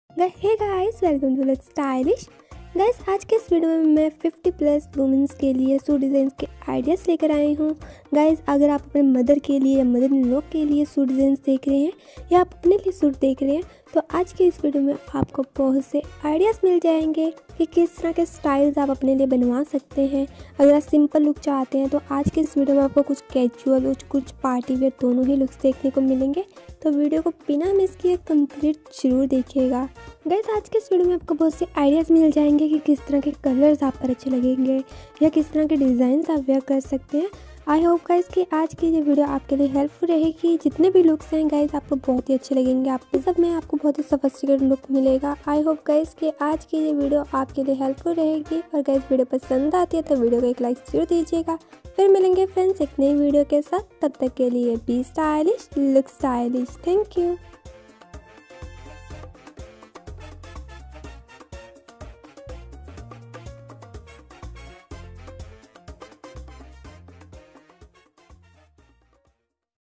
Suit Designs Ideas For 50+ Age Women's || Suits For Women Over 50 || by Look Stylish
Hello friends 😊

In today's video I share suit designs ideas for 50+ age women's, here you'll see different style of suit designs, I hope friends you like this video.